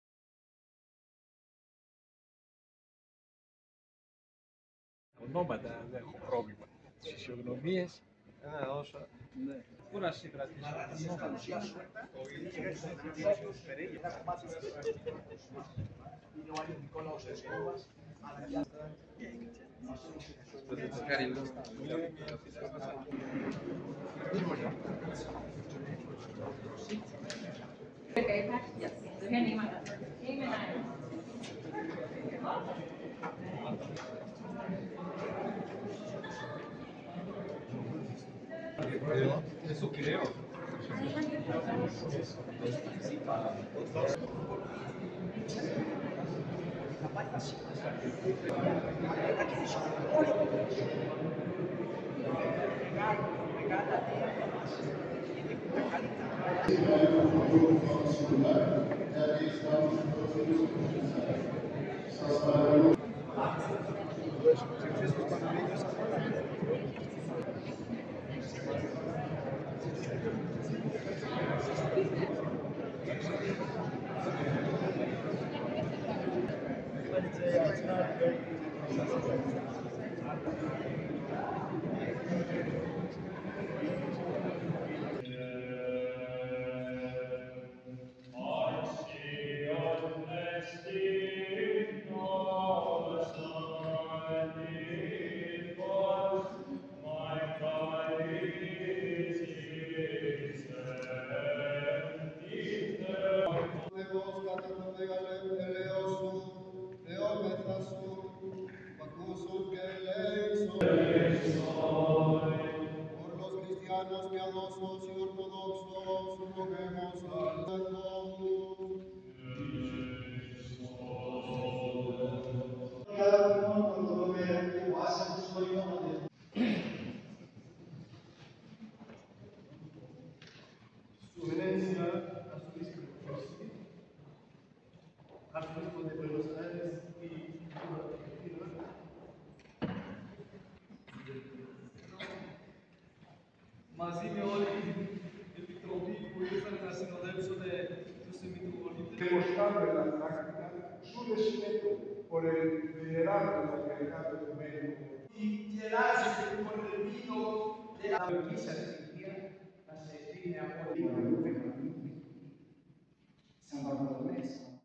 Iakovos Andriopoulus, nuevo Metropolitano de la Iglesia Ortodoxa Griega en México
Entronización de Iakovos Andriopoulus como nuevo jerarca Metropolitano de la Iglesia Ortodoxa Griega en México.
Iglesia de Santa Sofía
16-Mar-2023